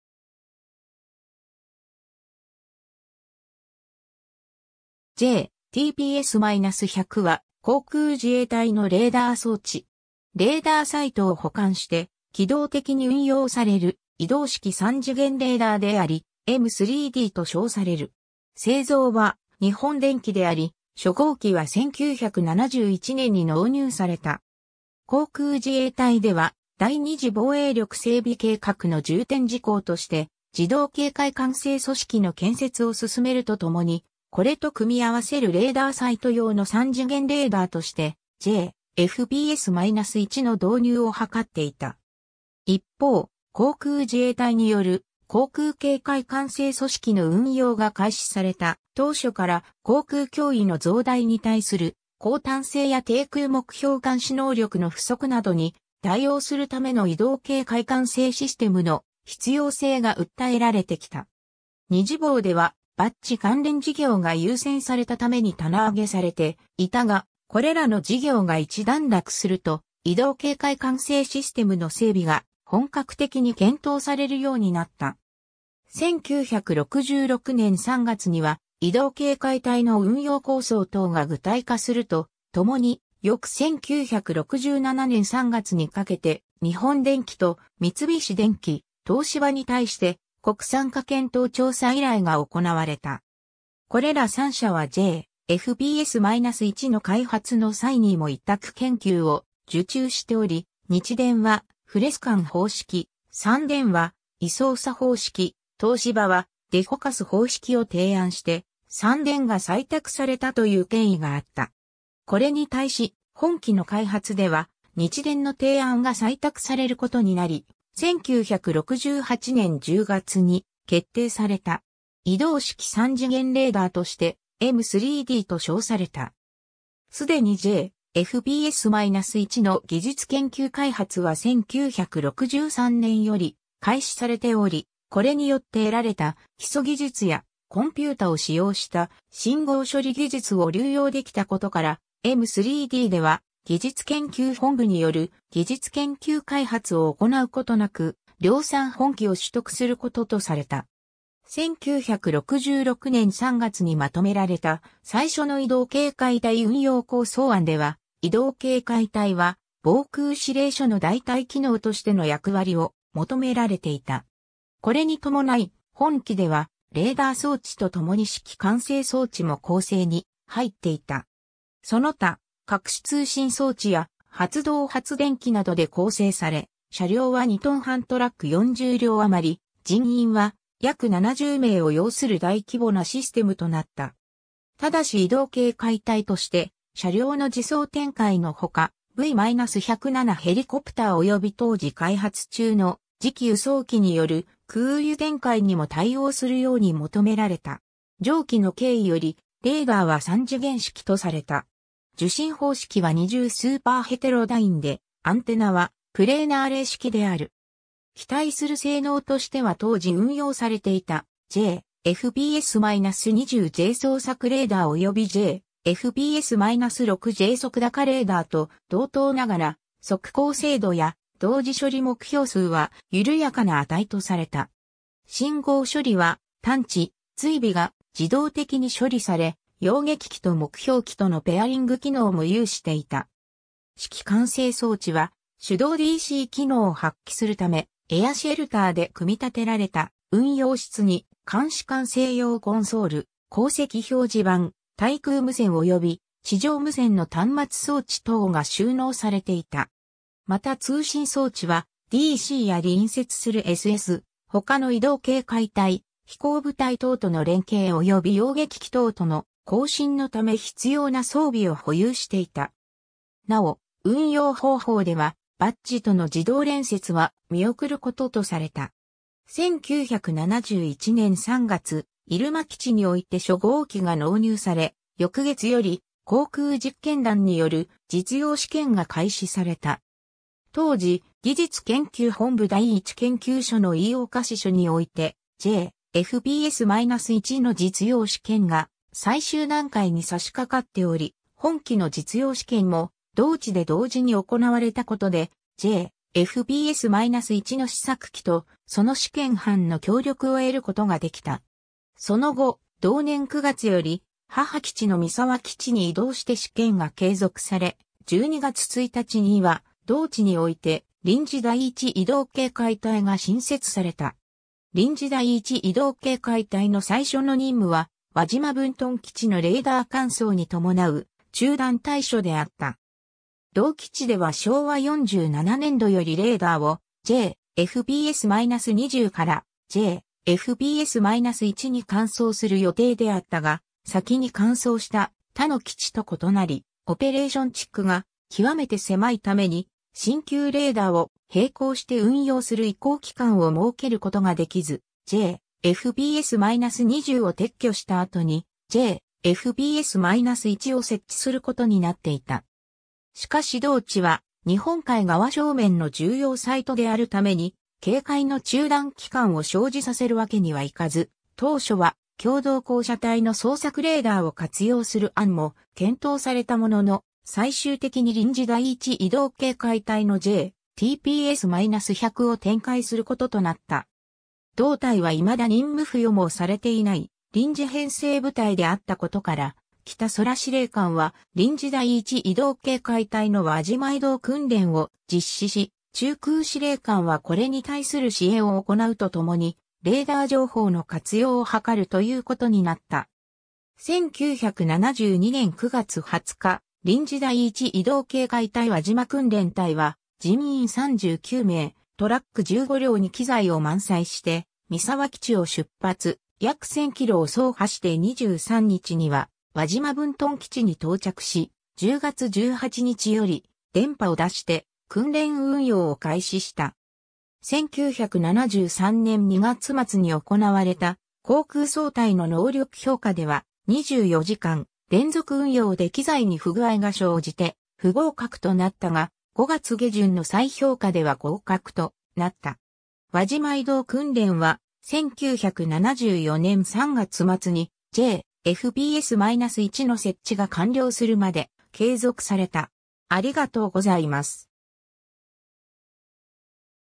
J/TPS-100
J/TPS-100は、航空自衛隊のレーダー装置。
レーダーサイトを補完して機動的に運用される移動式3次元レーダーであり、M-3Dと称される。
製造は日本電気であり、初号機は1971年に納入された。
 航空自衛隊では、第2次防衛力整備計画の重点事項として自動警戒管制組織（BADGE）の建設を進めるとともに、これと組み合わせるレーダーサイト用の3次元レーダーとしてJ/FPS-1（F-3D）の導入を図っていた。
一方、航空自衛隊による航空警戒管制組織の運用が開始された当初から、航空脅威の増大に対する抗堪性や低空目標監視能力の不足などに対応するための移動警戒管制システムの必要性が訴えられてきた。
2次防ではBADGE関連事業が優先されたために棚上げされていたが、これらの事業が一段落すると、移動警戒管制システムの整備が本格的に検討されるようになった。
 1966年3月には移動警戒隊の運用構想等が具体化するとともに、翌1967年3月にかけて、日本電気と三菱電機、東芝に対して、国産化検討調査依頼が行われた。
これら3社はJ/FPS-1の開発の際にも委託研究を受注しており、日電はFRESCAN方式、三電は位相差方式、東芝はDefocus方式を提案して、三電が採択されたという経緯があった。
これに対し、本機の開発では日電の提案が採択されることになり、1968年10月に決定された。
移動式3次元レーダーとして、M-3Dと称された。
既にJ/FPS-1（F-3D）の技術研究開発は1963年より開始されており、これによって得られた基礎技術や、コンピュータを使用した信号処理技術を流用できたことから、M-3Dでは技術研究本部による技術研究開発を行うことなく、量産本機を取得することとされた。
 1966年3月にまとめられた最初の移動警戒隊運用構想案では、移動警戒隊は防空指令所（DC）の代替機能としての役割を求められていた。
これに伴い、本機では、レーダー装置とともに指揮管制装置も構成に入っていた。
その他、各種通信装置や発動発電機（10台）などで構成され、車両は2トン半トラック40両余り、人員は約70名を要する大規模なシステムとなった。
ただし移動警戒隊として、車両の自走展開のほか、V-107ヘリコプターおよび当時開発中の次期輸送機（後のC-1）による空輸展開にも対応するように求められた。
 上記の経緯より、レーダーは3次元式とされた。
受信方式は2重スーパーヘテロダインで、アンテナはプレーナアレイ式である。
期待する性能としては当時運用されていたJ/FPS-20J捜索レーダーおよびJ/FPS-6J測高レーダーと同等ながら、測高精度や同時処理目標数は緩やかな値とされた。
信号処理は、探知・追尾が自動的に処理され、要撃機と目標機とのペアリング機能も有していた。
 指揮管制装置は、手動DC機能を発揮するため、エアシェルターで組み立てられた運用室に、監視管制用コンソール、航跡表示板、対空無線および地上無線の端末装置等が収納されていた。
また通信装置は、DCや隣接するSS、他の移動警戒隊、飛行部隊等との連携及び要撃機等との交信のため必要な装備を保有していた。
なお運用方法では、BADGEとの自動連接は見送ることとされた。
 1971年3月、入間基地において初号機が納入され、翌月より航空実験団による実用試験が開始された。
当時、技術研究本部第1研究所の飯岡支所においてJ/FPS-1の実用試験が最終段階に差し掛かっており、本機の実用試験も同地で同時に行われたことで、J/FPS-1の試作機とその試験班の協力を得ることができた。
その後、同年9月より母基地の三沢基地に移動して試験が継続され、12月1日には同地において臨時第1移動警戒隊が新設された。
 臨時第1移動警戒隊の最初の任務は、輪島分屯基地のレーダー換装に伴う中断対処であった。
同基地では昭和47年度よりレーダーをJ/FPS-20からJ/FPS-1に換装する予定であったが、先に換装した他の基地と異なり、オペレーション地区が極めて狭いために新旧レーダーを並行して運用する移行期間を設けることができず、J/FPS-20を撤去した跡にJ/FPS-1を設置することになっていた。
しかし同地は日本海側正面の重要サイトであるために警戒の中断期間を生じさせる訳にはいかず、当初は教導高射隊の捜索レーダーを活用する案も検討されたものの、最終的に臨時第1移動警戒隊のJ/TPS-100を展開することとなった。
同隊は未だ任務付与も...